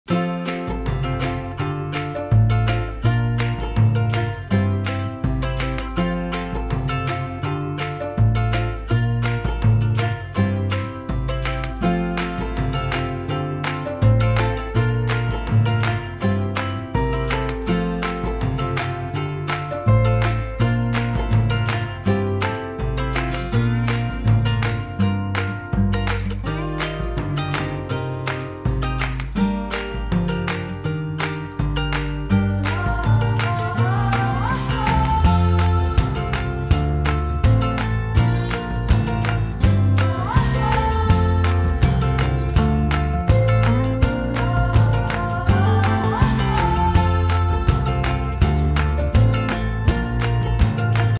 Spring Bird Craft For Kids
Spring Crafts | Kids Crafts

In this video, we're diving into the world of spring crafts with an adorable and easy DIY bird craft for kids. Watch along as we guide you through each step of creating your very own colorful spring bird using simple materials you likely already have at home.

This spring bird craft is not only a fun activity for your little ones, but it also helps develop their creativity and fine motor skills. So grab your crafting supplies and join us as we bring a little bit of nature indoors with this delightful spring bird craft.